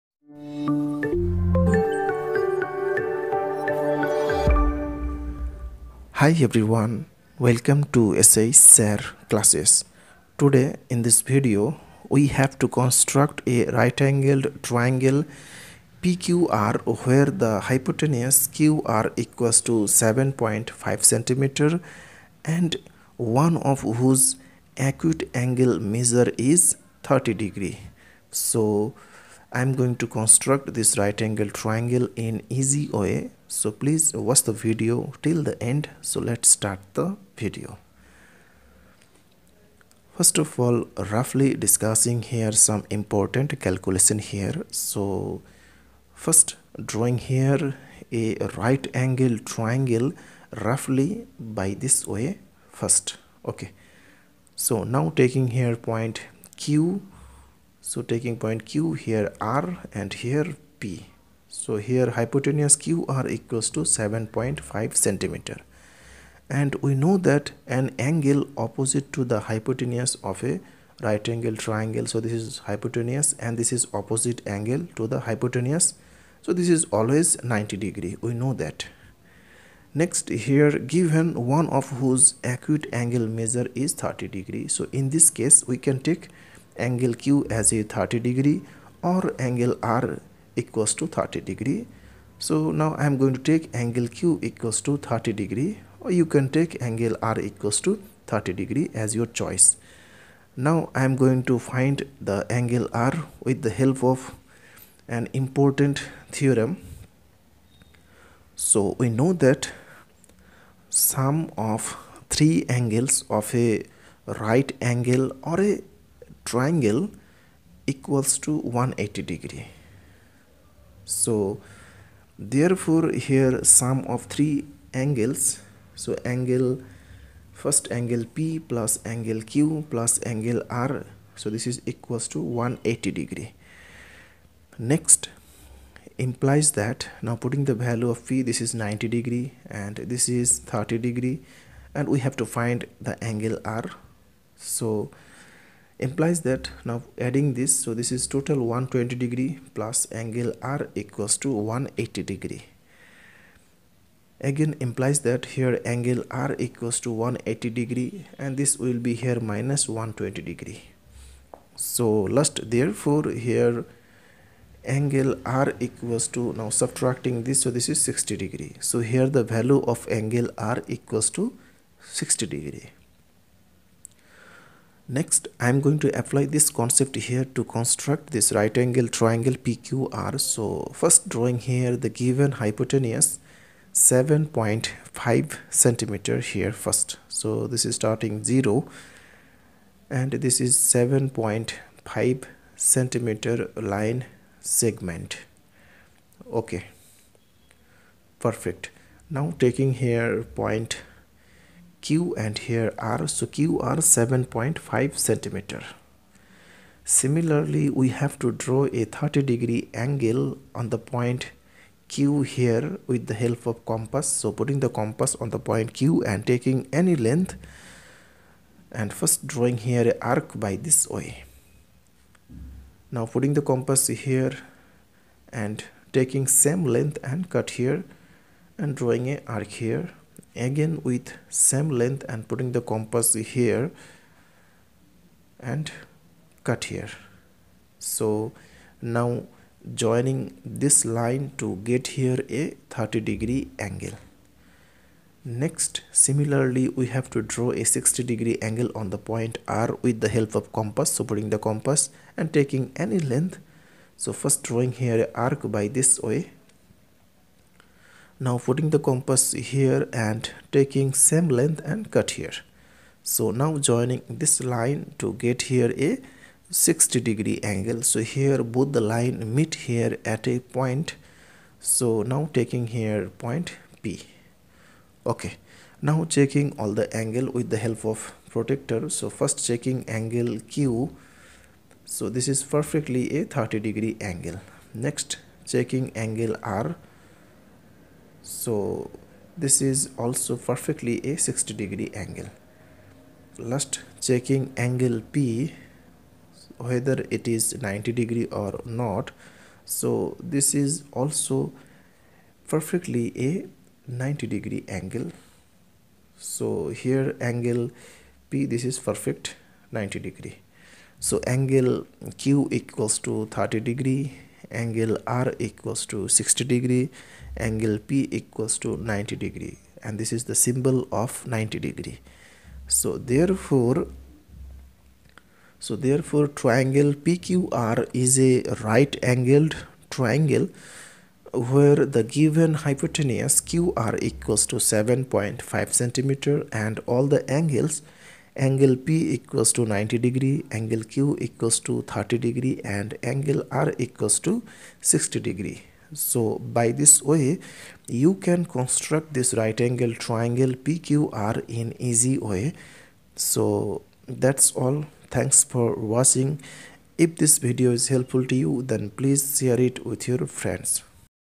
Construct a right angle triangle where hypotenuse QR=7.5cm and one of whose acute angle 30°.
In Easy Way how to construct a right angle triangle where hypotenuse QR=7.5cm and one of whose acute angle measure 30°.@SHSIRCLASSES. Geometrical Construction.